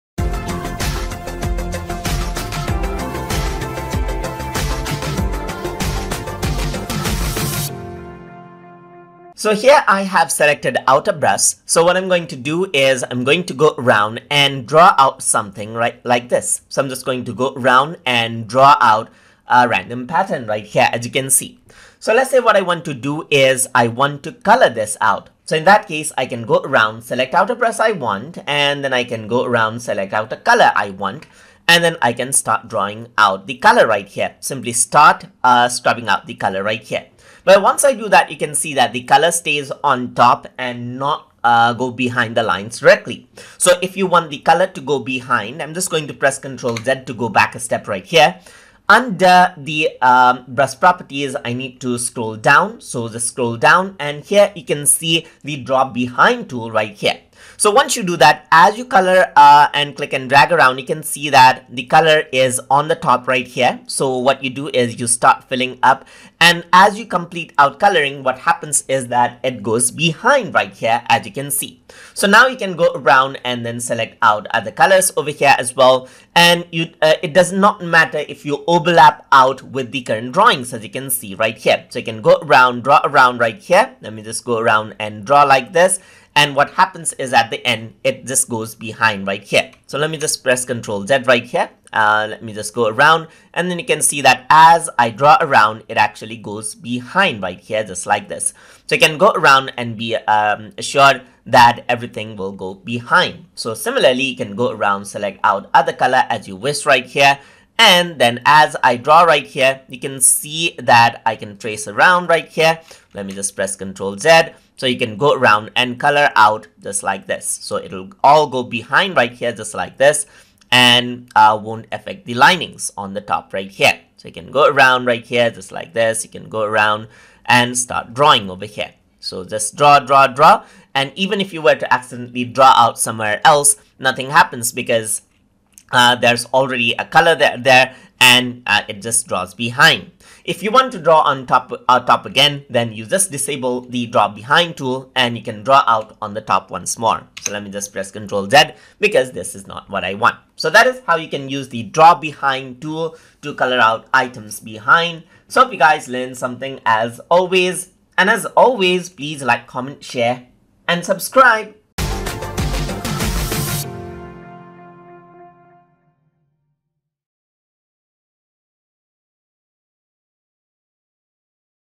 Toon Boom Storyboard Pro Tutorial - Lesson 41 - Draw Behind Tool
In this tutorial, we will be discussing about Draw Behind Tool in Toon Boom Storyboard Pro.